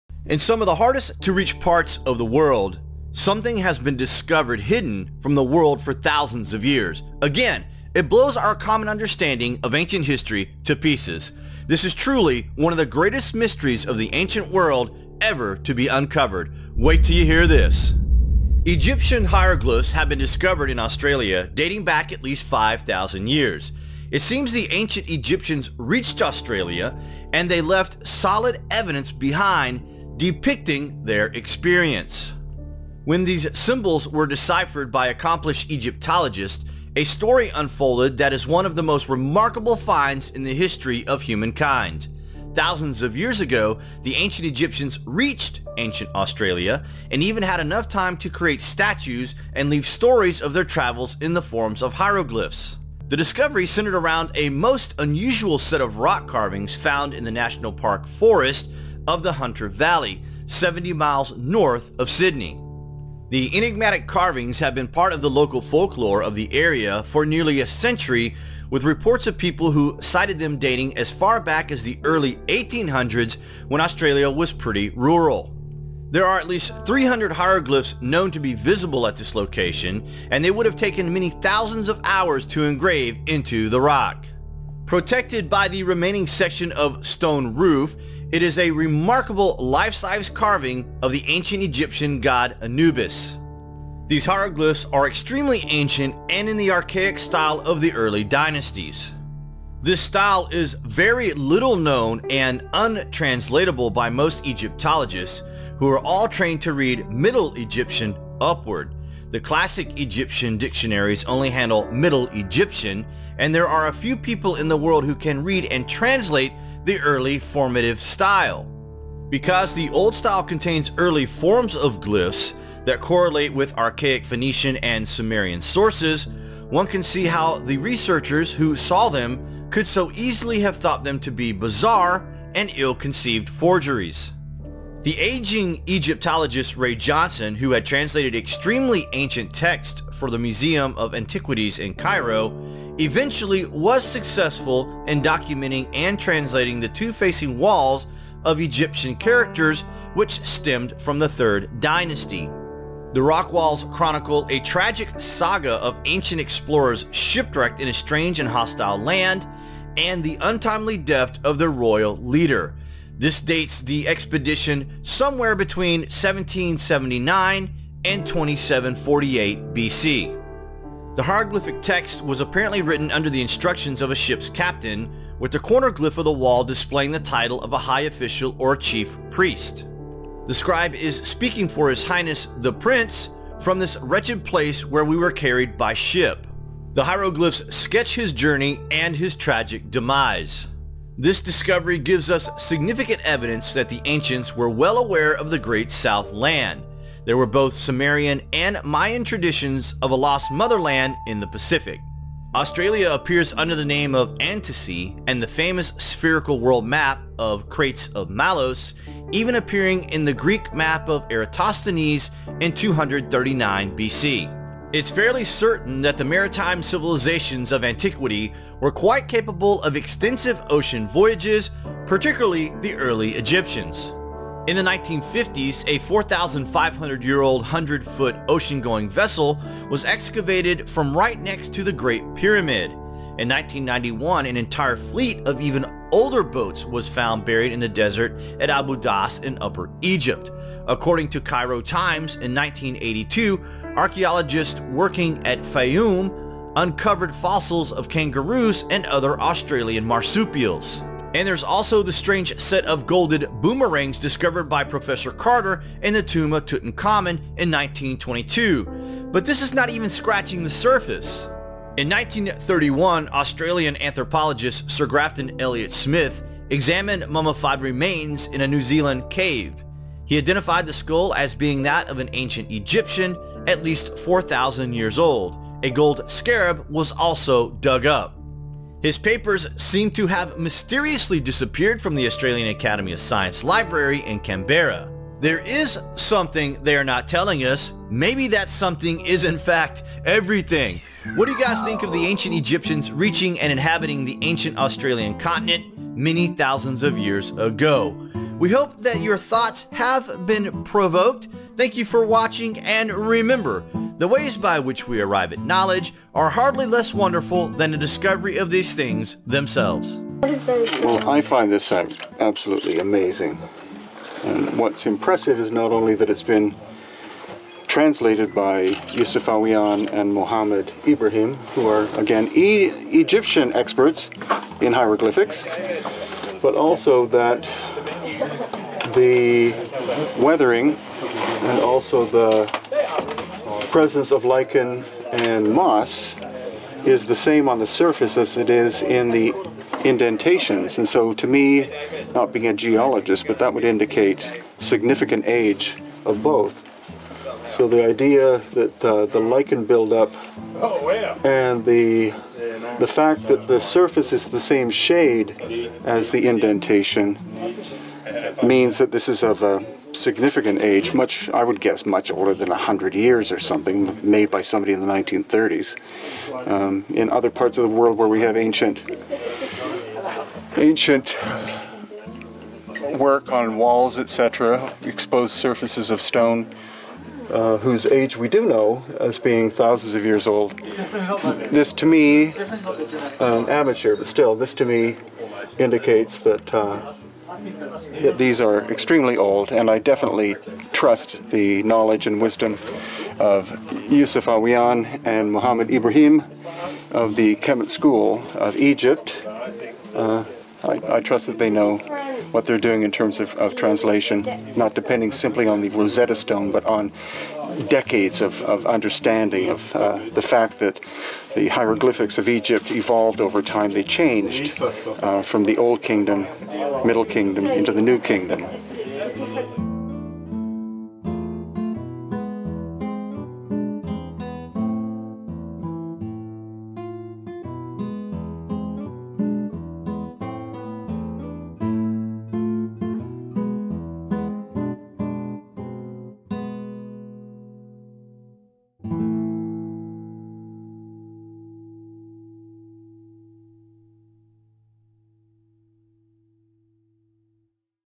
Ancient Egypt in Australia?! (PART 1)
The discovery centers around a most unusual set of rock carvings found in the National Park forest of the Hunter Valley, 70 miles north of Sydney.
The enigmatic carvings have been part of the local folklore of the area for nearly a century with reports of people who sighted them dating as far back as the early 1800′s when Australia was pretty rural.
There are at least 300 hieroglyphs known to be visible at this location and they would have taken many thousands of hours to engrave into the rock.